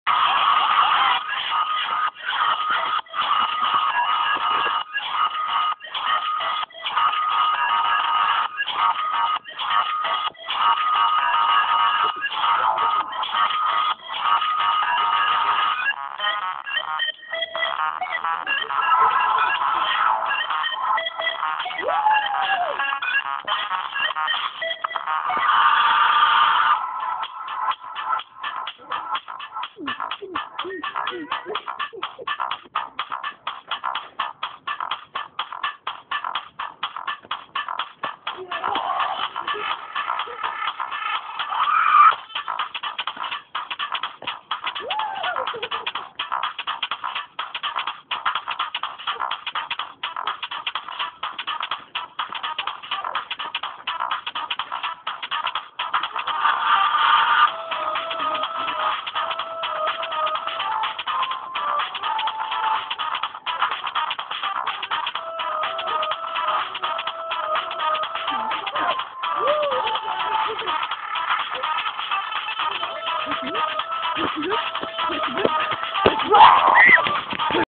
drei deppen ohne sinn für humor und total crank xD
erste versuche jumpstyle zu tanzen :D
steffi total gestört xD